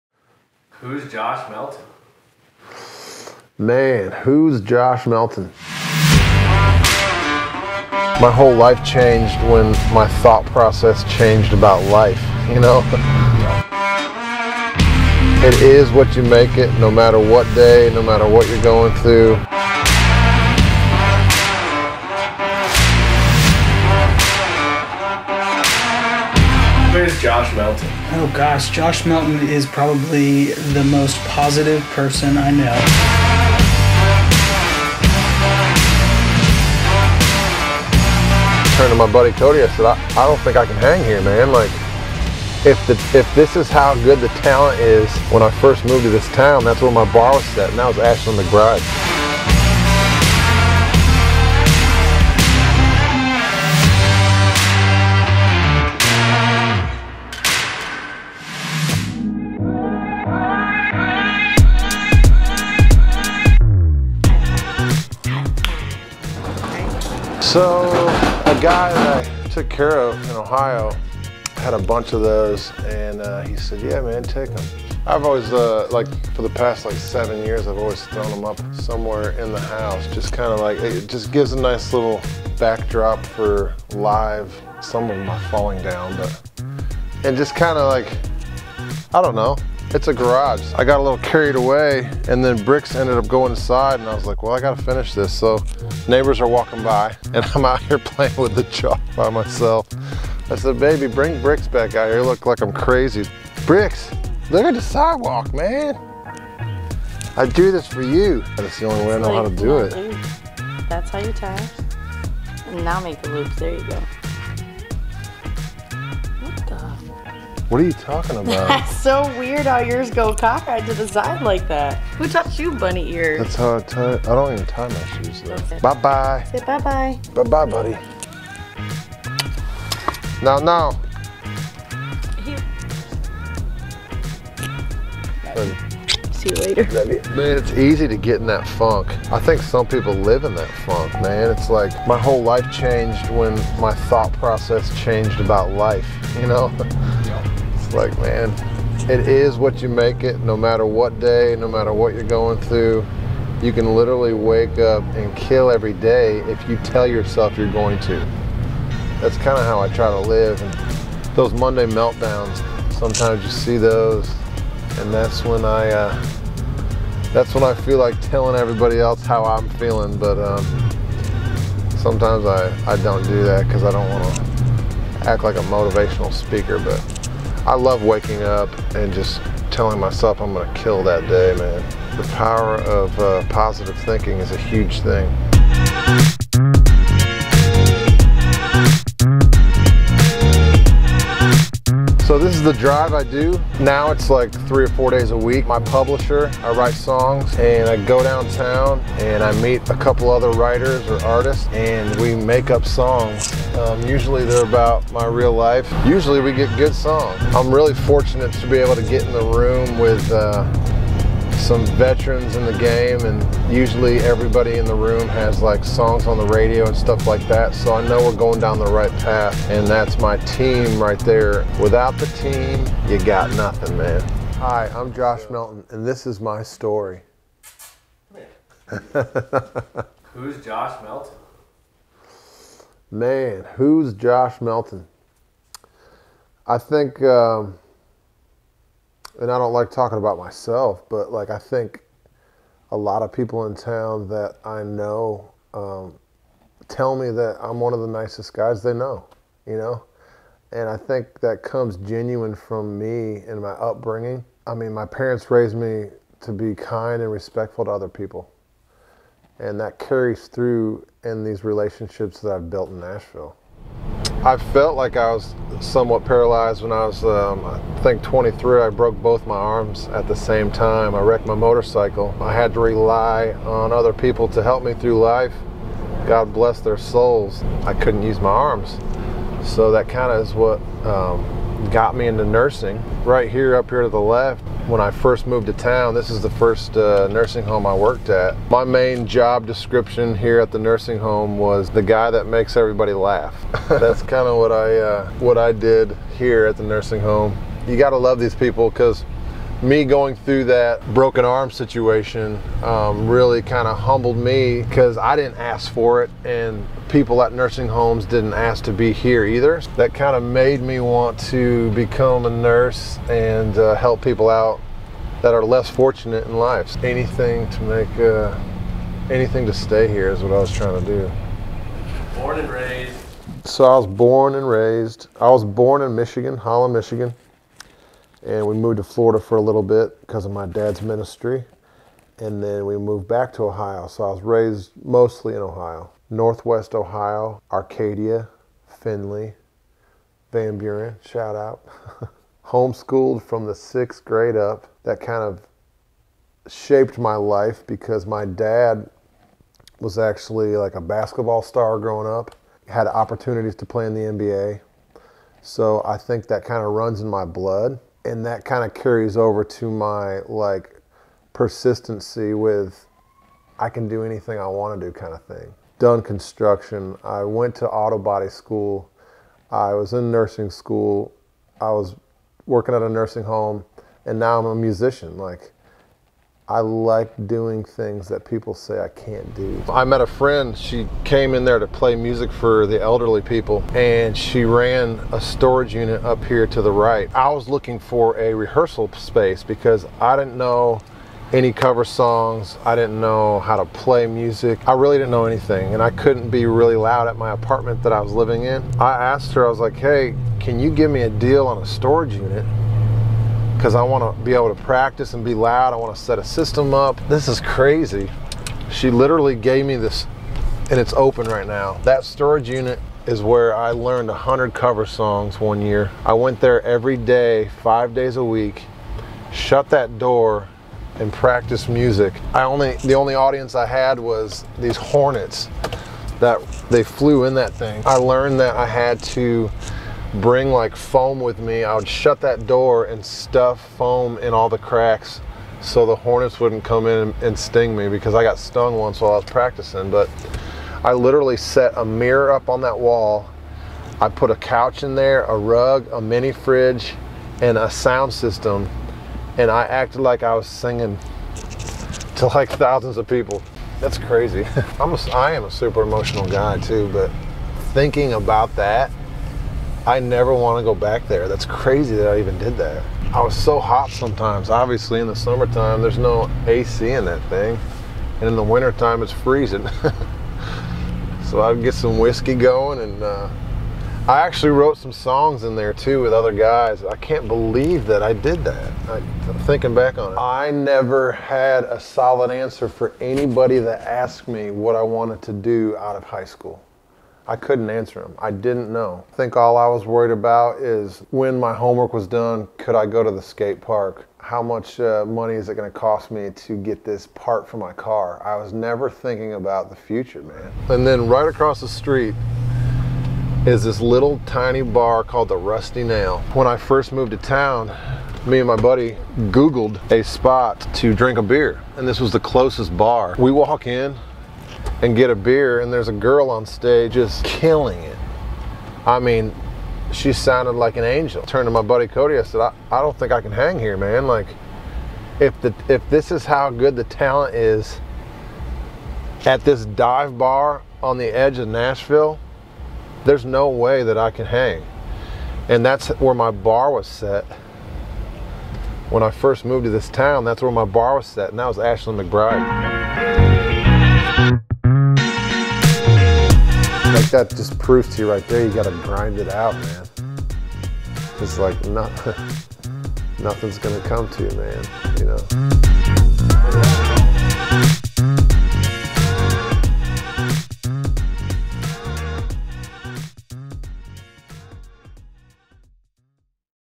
Who Is Josh Melton || Episode 1 - Finding His Way To Nashville
Who is Josh Melton? A mini-docuseries introducing the world to up and coming Nashville based country artist, Josh Melton. Ohio born and raised, Josh shares his story of his journey to Nashville and what it has taken to get this far.